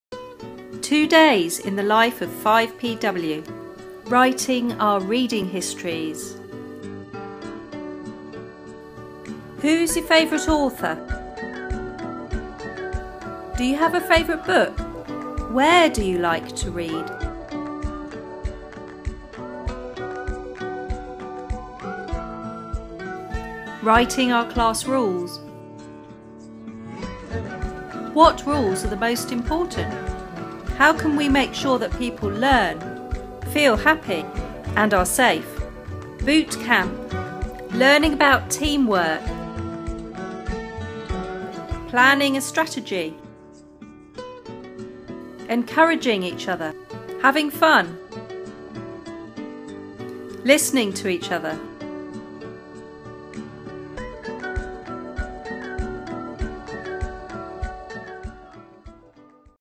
Two Days In 5PW
Mrs Woodhouse explains what her 5PW class have been learning during the first two days of the new school year.